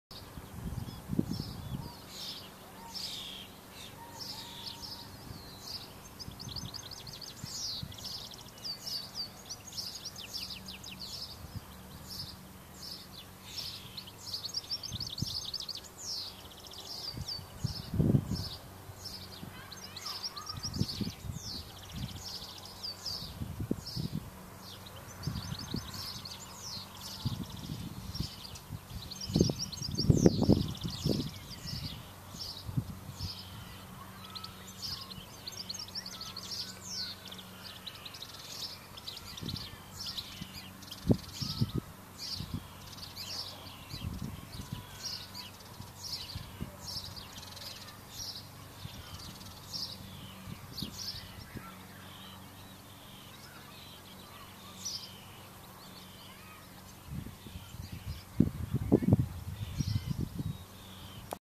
Autumn Trees Yellow Field Newbigging Scotland
Tour Scotland Autumn video of trees in a field of yellow on visit to Newbigging.